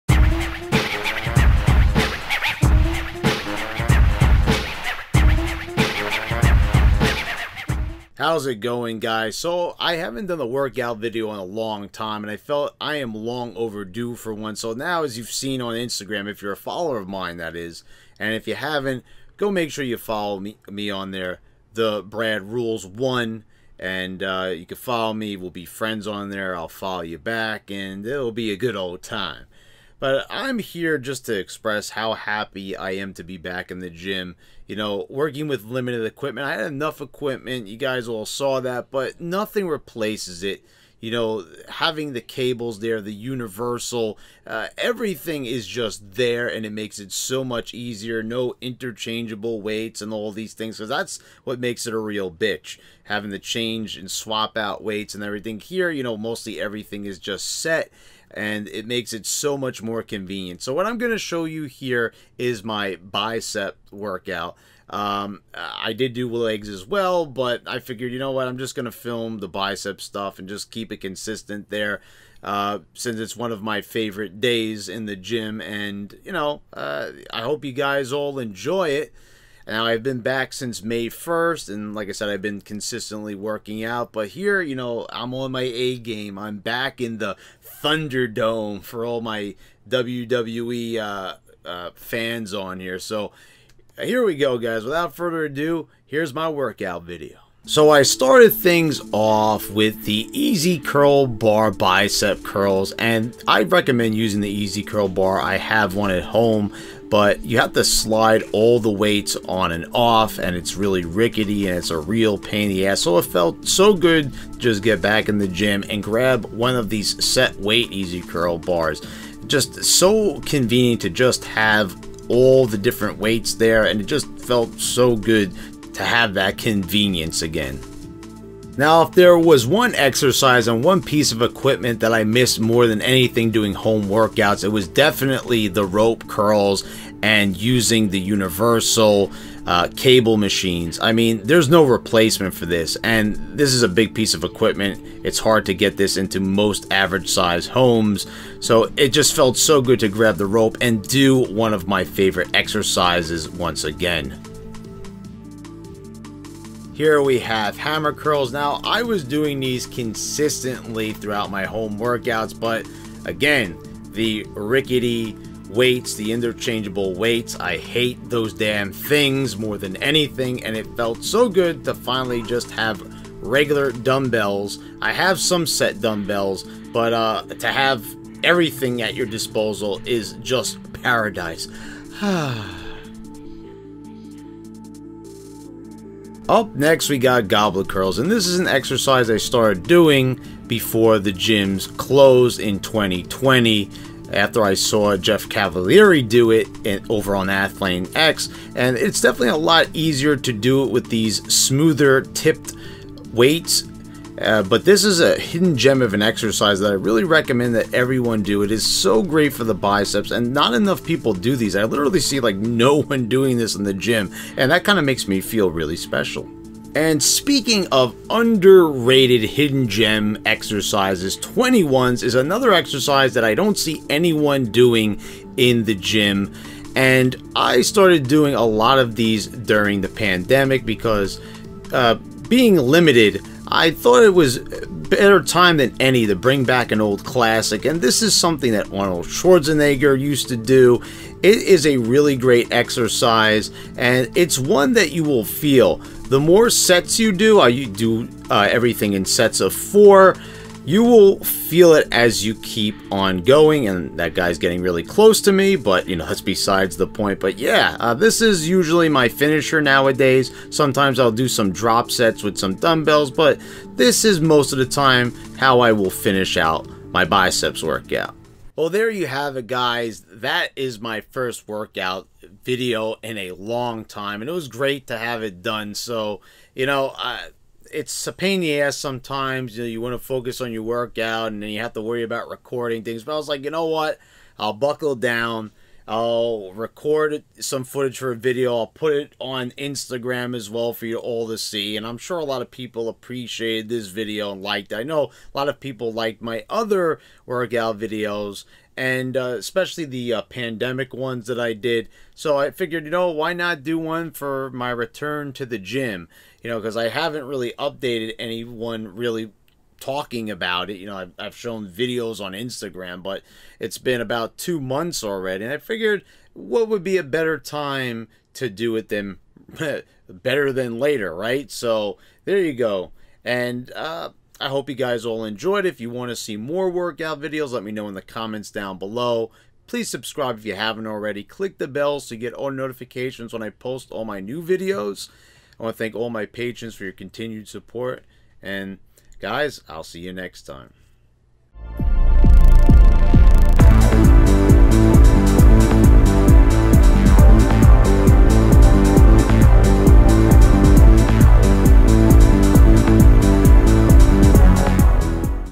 My Return to the Gym!
It's been a while since I've done a workout video, and what a better way to jump back into it, then by chronicling my return to the gym!